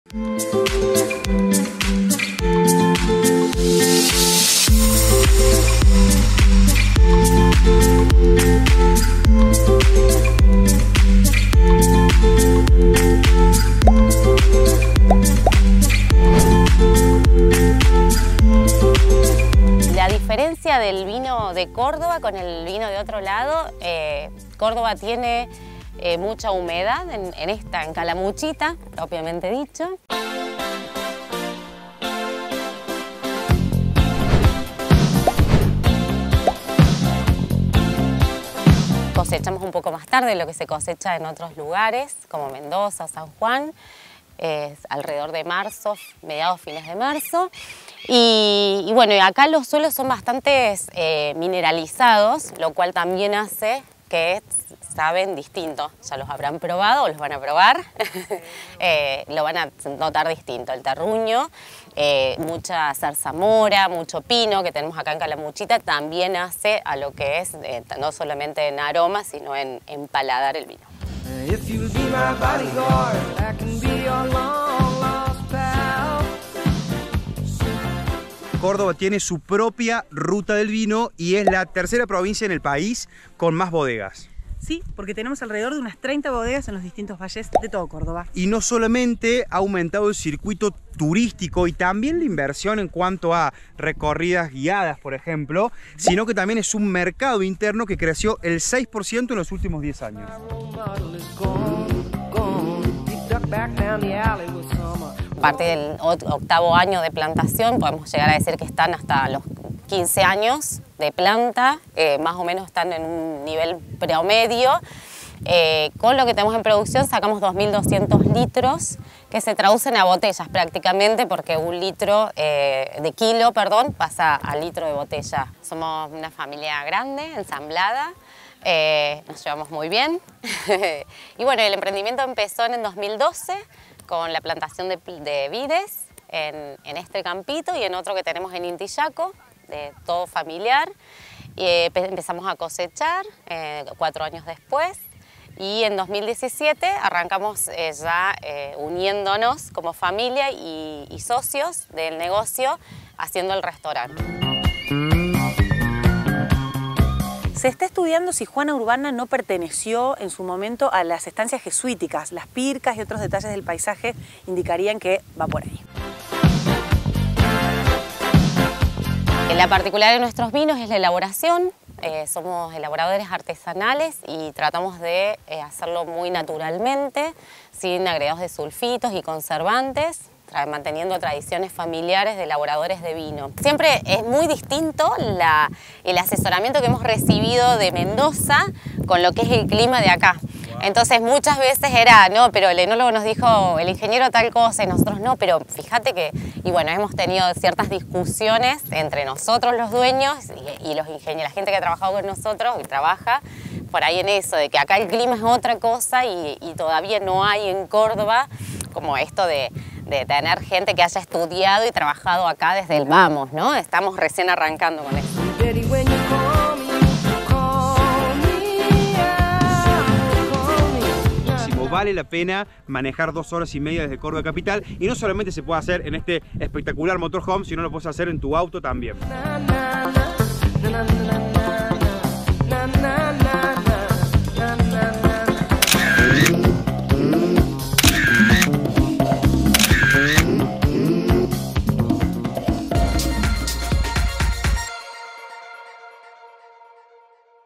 Los Marianes: recorremos la ruta del vino cordobés
En el Día Nacional del Vino, la dupla de Mariano Cardarelli y Mariana Asan salieron en motorhome a recorrer una bodega para conocer más de los vinos artesanales de Villa Yacanto. En Córdoba hay alrededor de 30 bodegas y el circuito turístico del vino no deja de crecer.